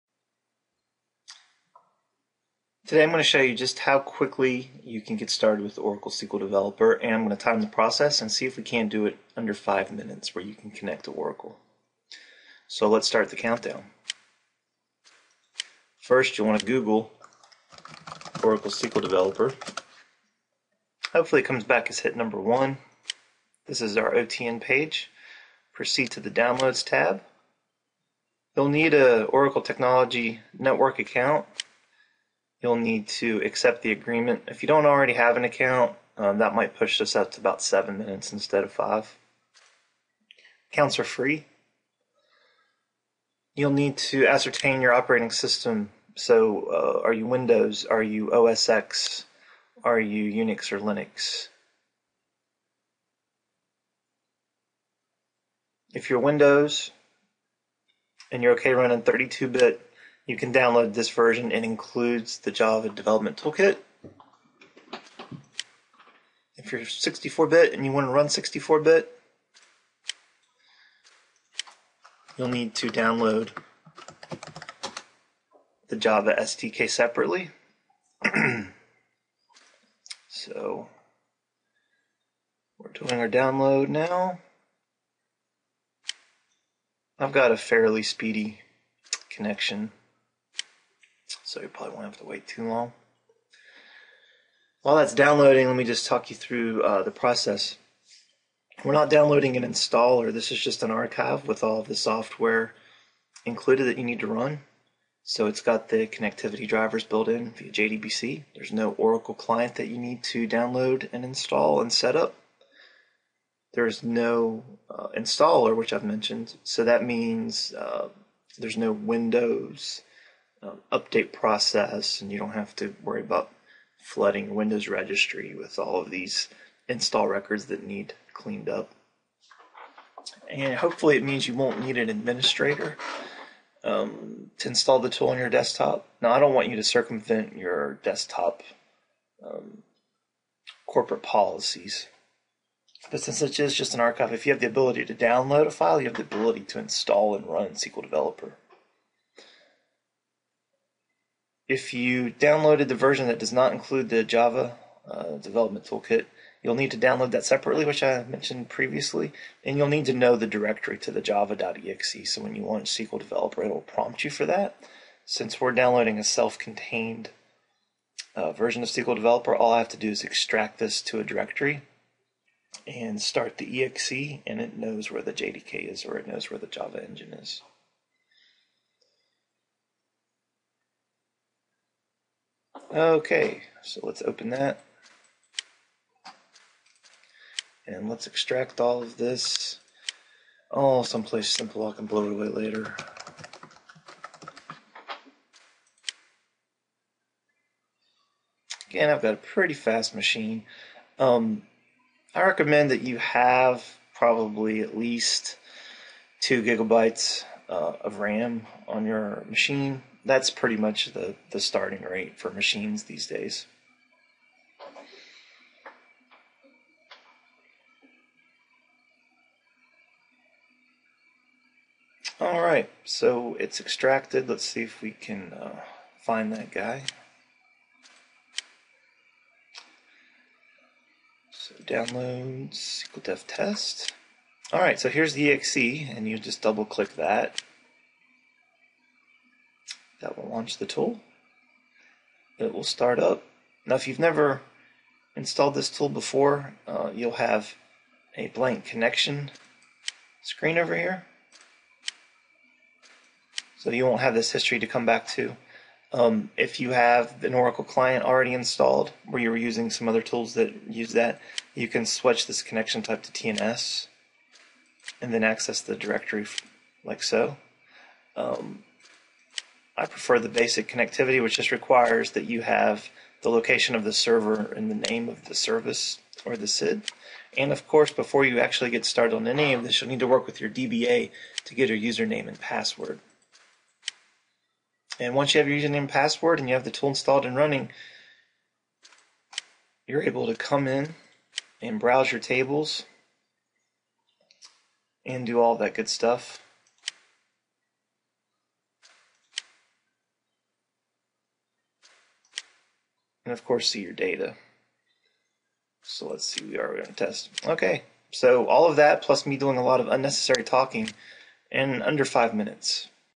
Connect to Oracle in less than 5 minutes with SQL Developer
Oracle SQL Developer Product Manager Jeff Smith shows you how easy it is to get started with the database integrated development environment (IDE). Download, extract, start, and connect in under 5 minutes!